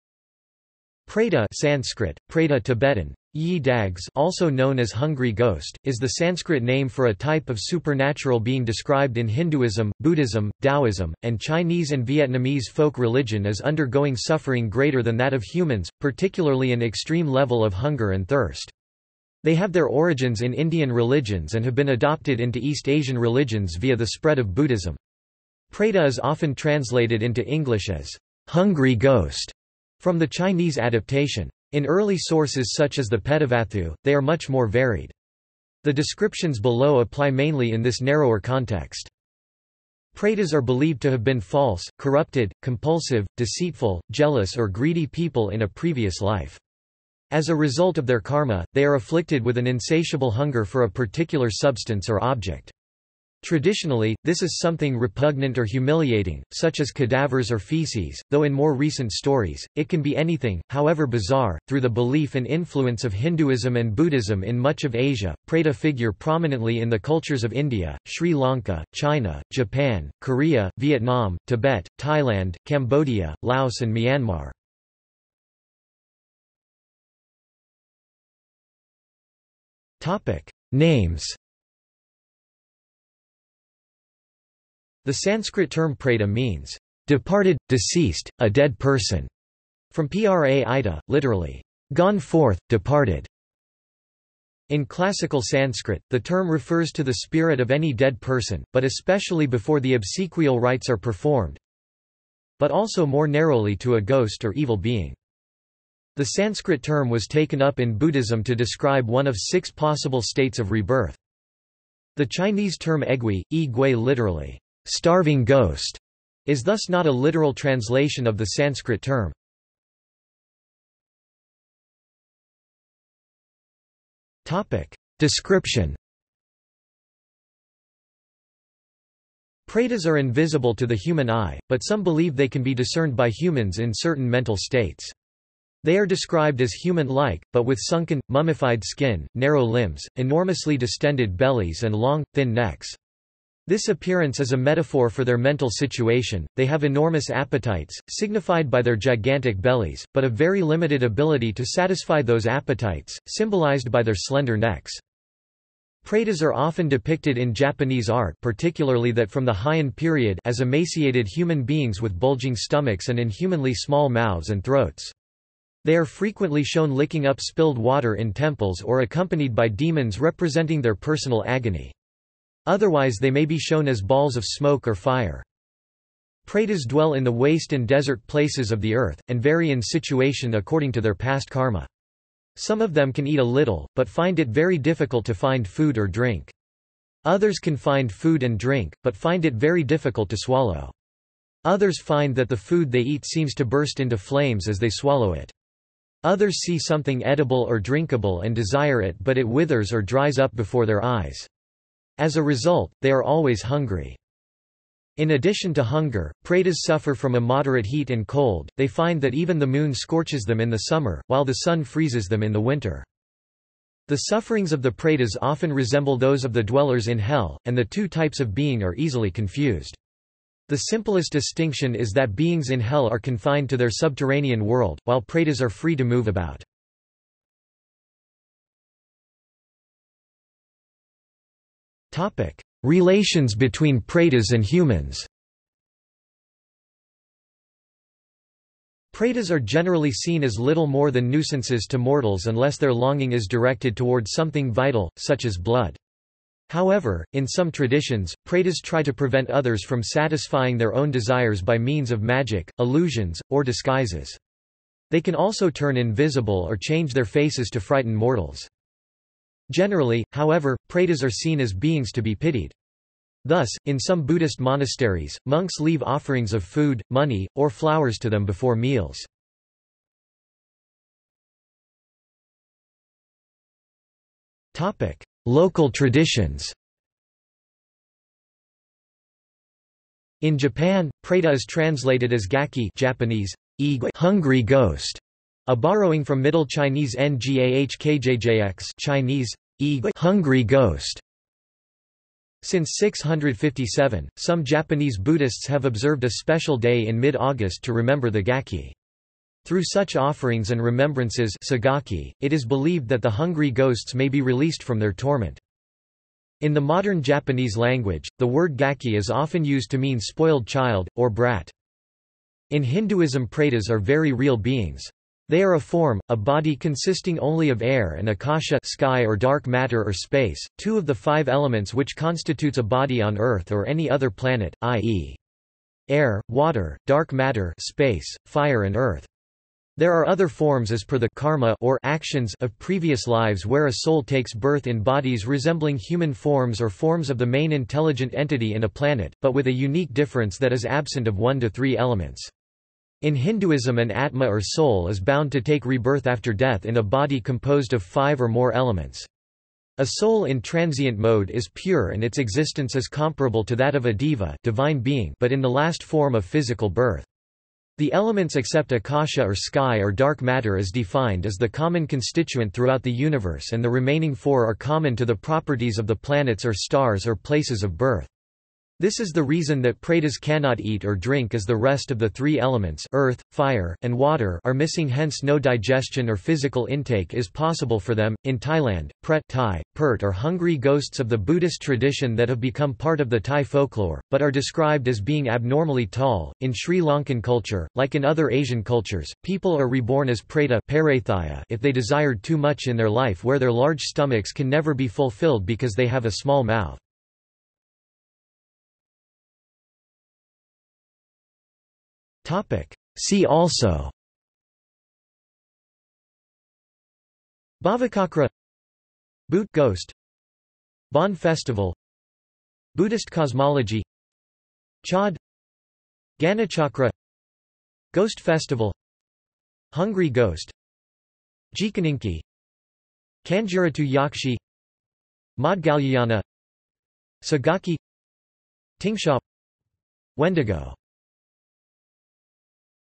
Preta | Wikipedia audio article
This is an audio version of the Wikipedia Article:
Preta

00:01:16 1 Names
00:02:00 2 Description
00:03:48 3 Relations between pretas and humans
00:04:31 4 Local traditions
00:06:38 5 See also

- Socrates

SUMMARY
Preta (Sanskrit: प्रेत),(Tibetan: ཡི་དྭགས་ (yi dags)) also known as hungry ghost, is the Sanskrit name for a type of supernatural being described in Hinduism, Buddhism, Taoism, and Chinese and Vietnamese folk religion as undergoing suffering greater than that of humans, particularly an extreme level of hunger and thirst. They have their origins in Indian religions and have been adopted into East Asian religions via the spread of Buddhism. Preta is often translated into English as "hungry ghost" from the Chinese adaptation. In early sources such as the Petavatthu, they are much more varied. The descriptions below apply mainly in this narrower context.
Pretas are believed to have been false, corrupted, compulsive, deceitful, jealous or greedy people in a previous life. As a result of their karma, they are afflicted with an insatiable hunger for a particular substance or object. Traditionally, this is something repugnant or humiliating, such as cadavers or feces, though in more recent stories, it can be anything, however bizarre.Through the belief and influence of Hinduism and Buddhism in much of Asia, preta figure prominently in the cultures of India, Sri Lanka, China, Japan, Korea, Vietnam, Tibet, Thailand, Cambodia, Laos and Myanmar.